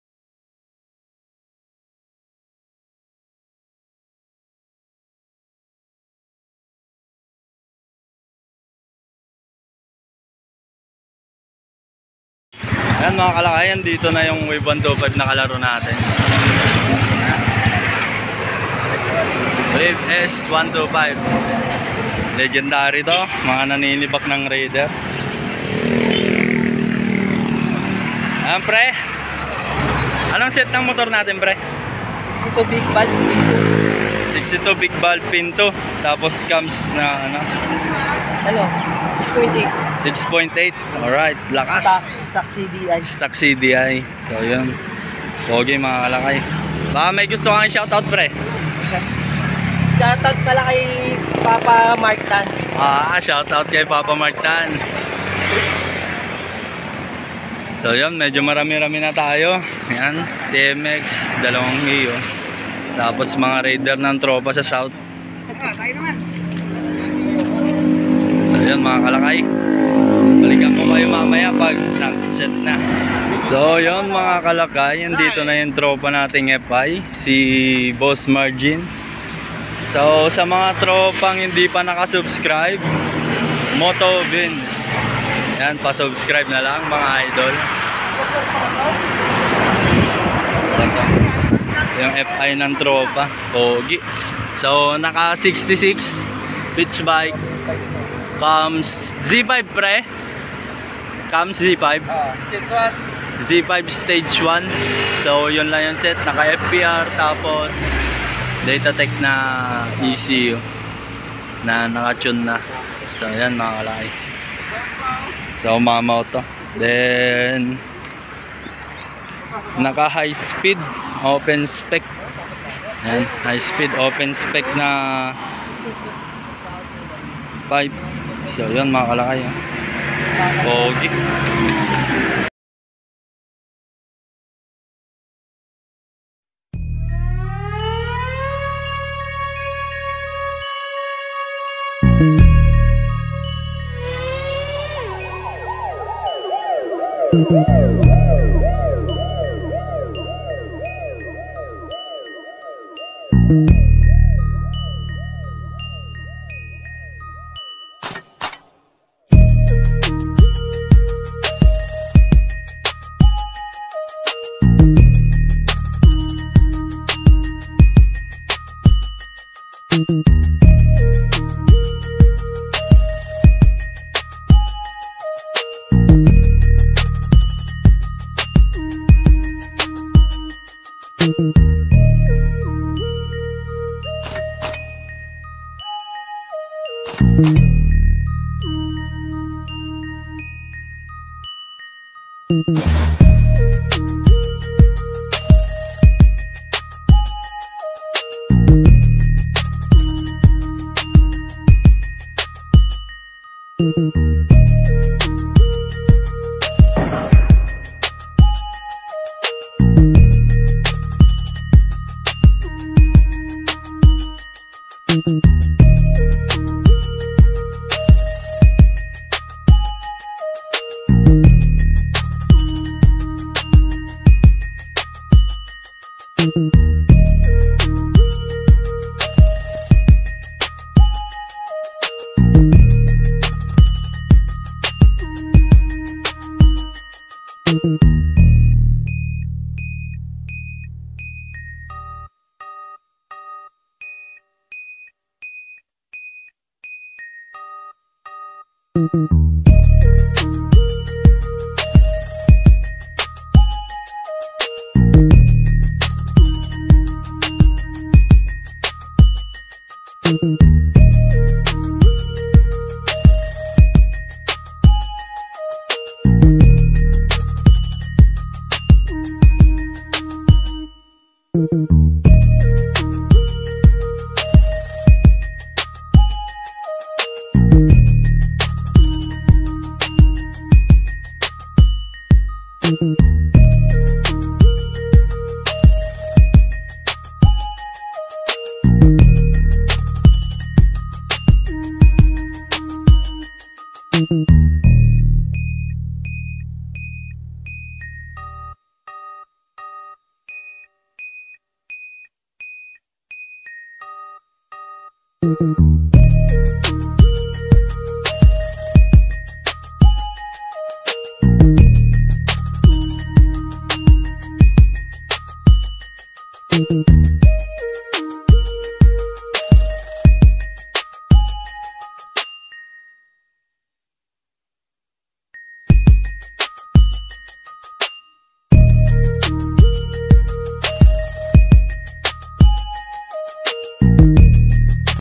raider 150 vs wave s 125 vs raider fi. KING DRAG PIPE PERFORMANCE
wave 125
62bore
pin 2
big valve
cams 6.8

raider 180cc cam holder

raider 150 pcc (black)

raider 180cc
gen1 blue

raider 206 black last video

raiderfi
66bore
z5 cams stage1

drag of underbone